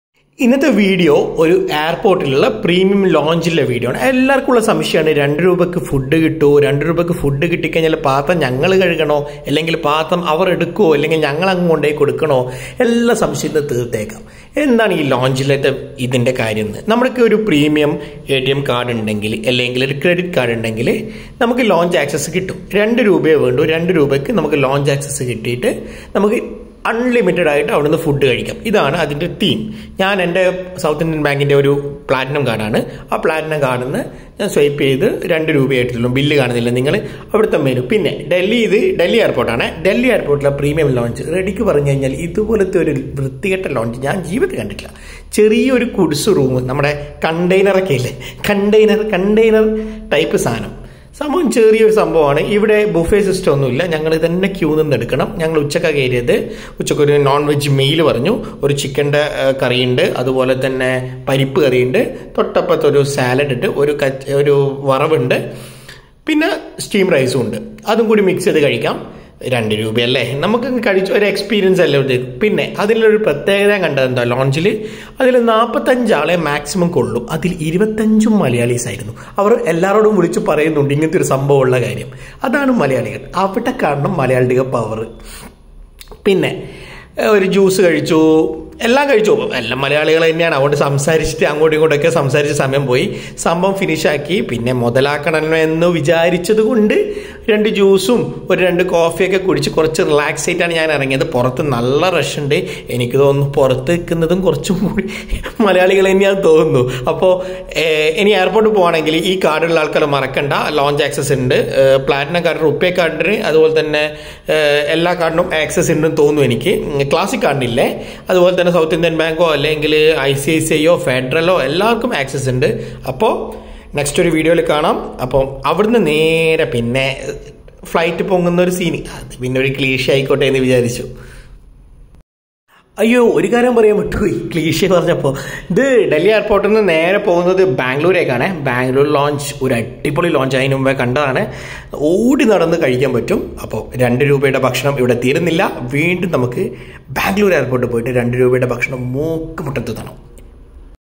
രണ്ടുരൂപക്ക് വയറുനിറയെ ഭക്ഷണം അതും എയർപോർട്ട് ലോഞ്ചിൽ . ഒന്നു കഴിച്ചുനോക്കുന്നോ ?
Indira Gandhi International Airport in Delhi
Terminal -2